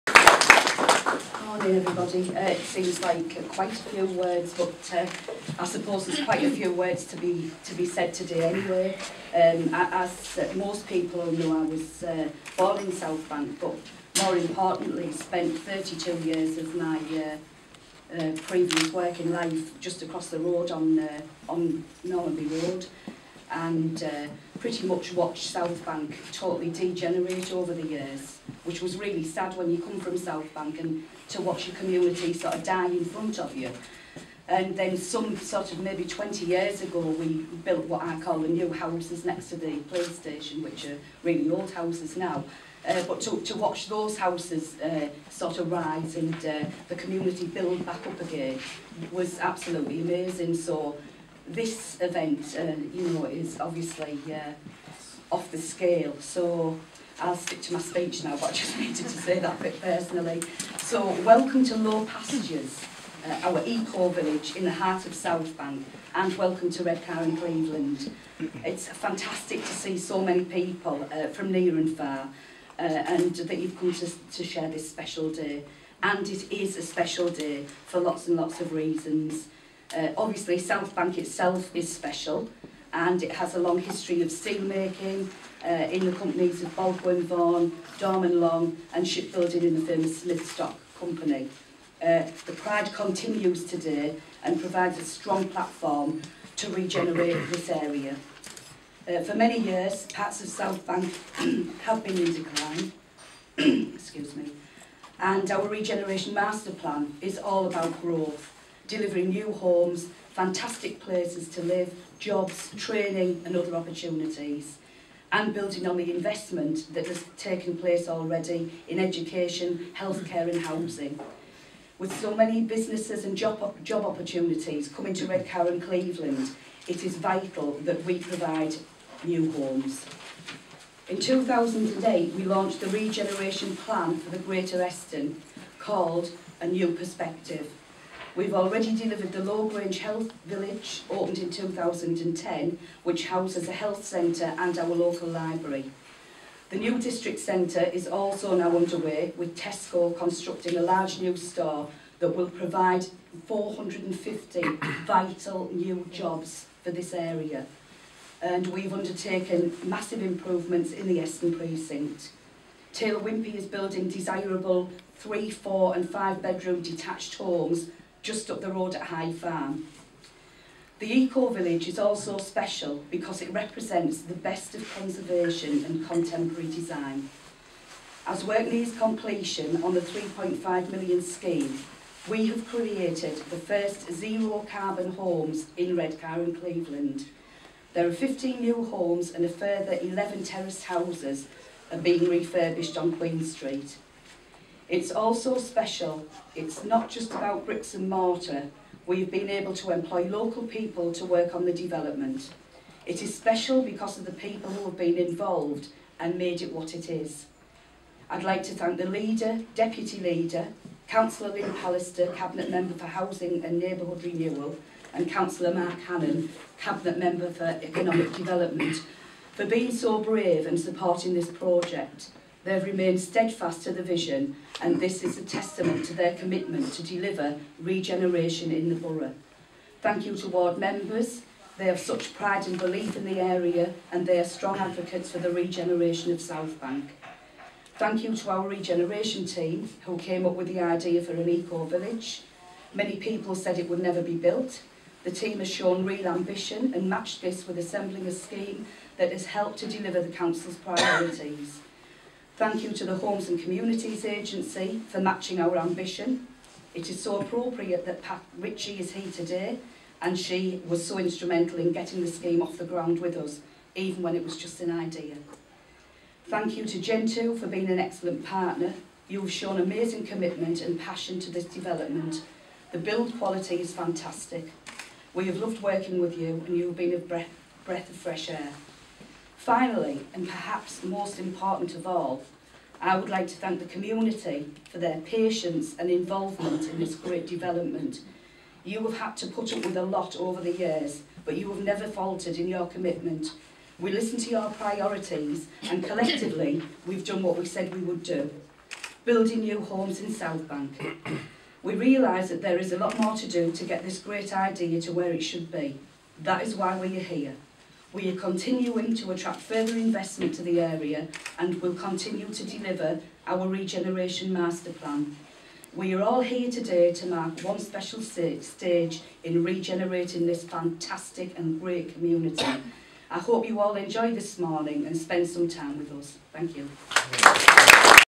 Eco Village in South Bank
Cllr Olwyn Peters, Mayor of Redcar & Cleveland Borough Council giving  the opening speech.

RESIDENTS of a community which has seen hundreds of houses demolished in recent times have celebrated the unveiling of its first new home in 20 years.

A stunning show home, featuring the latest environmentally-friendly technology, was officially opened at the ground-breaking £3.5m Eco Village in South Bank, on Teesside, on Friday, January 27.

And local people who have strongly supported the development were among the guests of honour when the showpiece property was revealed.